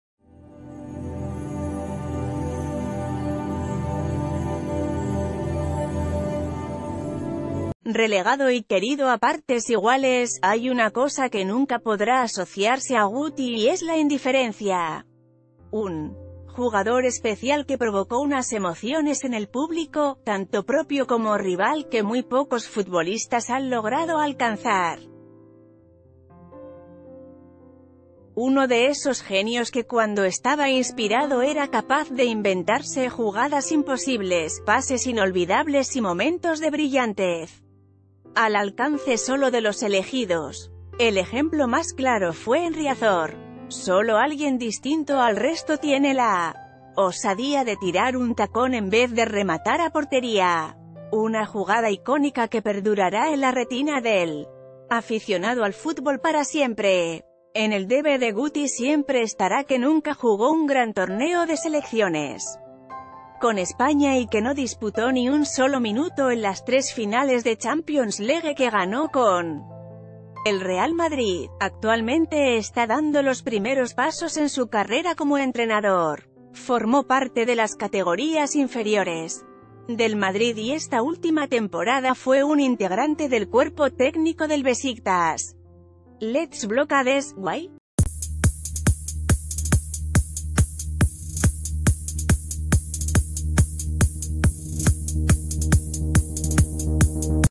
El gráfico de Guti, el futbolista que no dejó indiferente a nadie
Relegado y querido a partes iguales, hay una cosa que nunca podrá asociarse a Guti y es la indiferencia. Un jugador especial que provocó unas emociones en el público, tanto propio como rival, que muy ...